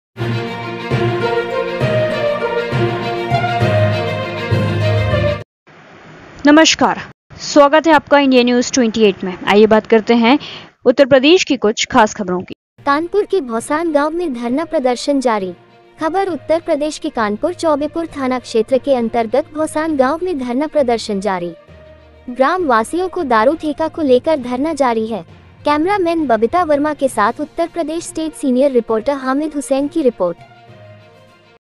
कानपुर के भौसान गांव में धरना प्रदर्शन जारी
Official